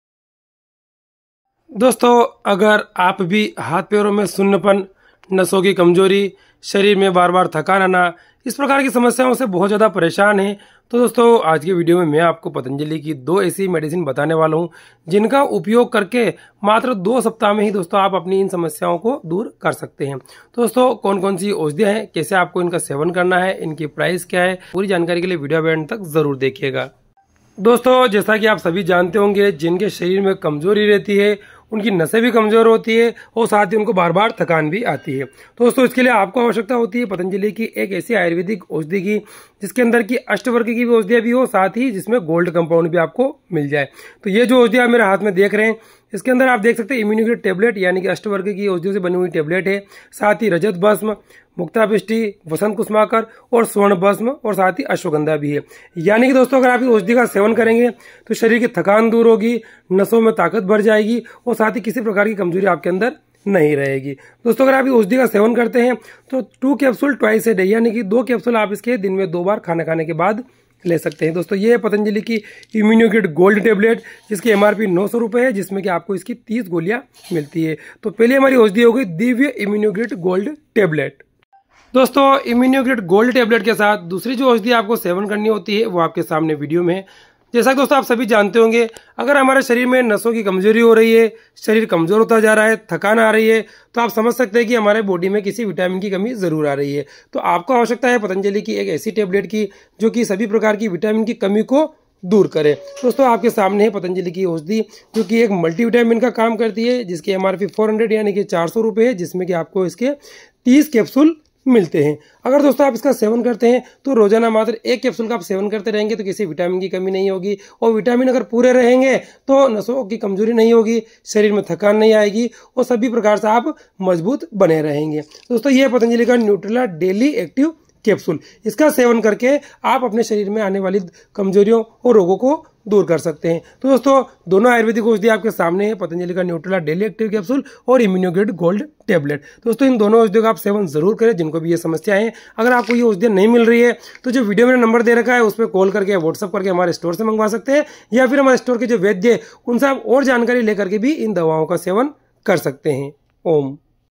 नसों की कमजोरी,थकान और हाथ - पैर सुन्न होना 2 हफ्ते मे जड़ से खत्म। TOP 2 MEDICINE FOR NEURO PROBLEM
नसों की कमजोरी,थकान और हाथ - पैर सुन्न होना 2 हफ्ते मे जड़ से खत्म। TOP 2 MEDICINE FOR NEURO PROBLEM @AyurvedStation